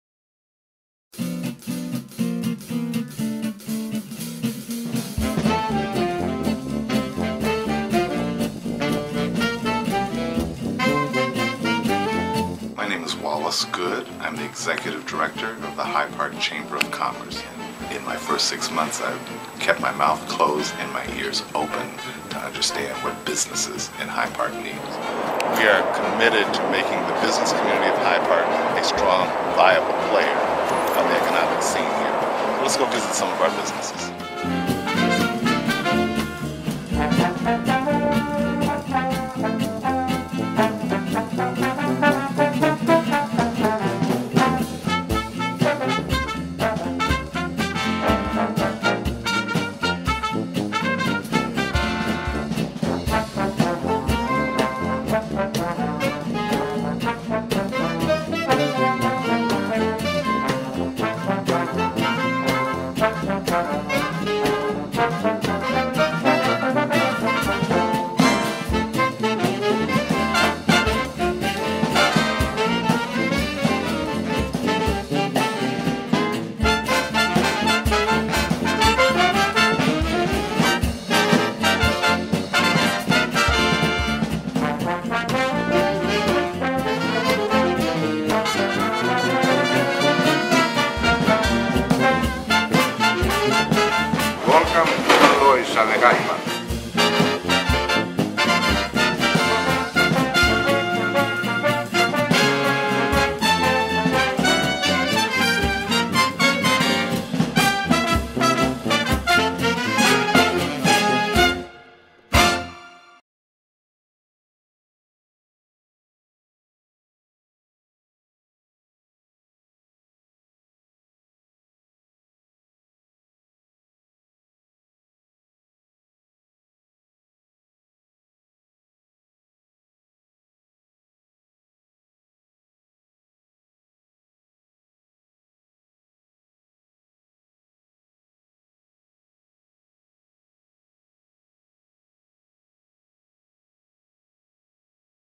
A Day in the Life of the Hyde Park Chamber of Commerce (ViolenTruth.com)
The Executive Director of the Hyde Park Chamber of Commerce, Wallace E. Goode, Jr. takes us to several businesses in the Hyde Park area to show you what they have to offer. Video Directed by Samariah Smith @ ViolenTruth.com Marketing. Filmed and Edited by Chan C. Smith.